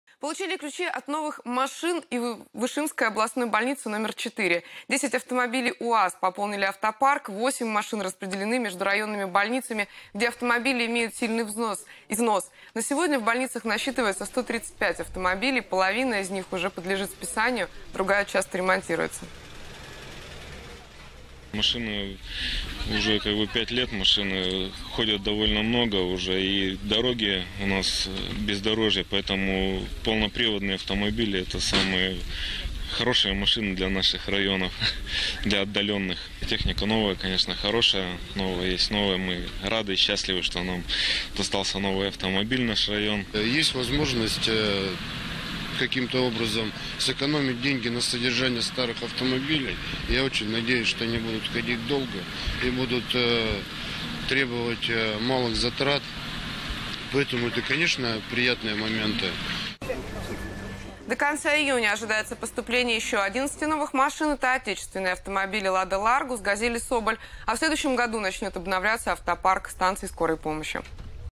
Ключи от новых машин
Ключи от новых машин получили в областной больнице № 4 в Ишиме. 10 автомобилей УАЗ пополнили автопарк. Восемь машин распределёны между районными больницами, где автомобили имеют сильный износ. На сегодня в больницах насчитывается 135 автомобилей, половина из них уже подлежит списанию, другая часто ремонтируются. о конца июня ожидается поступление еще одиннадцати новых машин – это отечественные автомобили Лада Ларгус, Газель и Соболь. А в следующем году начнёт обновляться автопарк станций скорой помощи.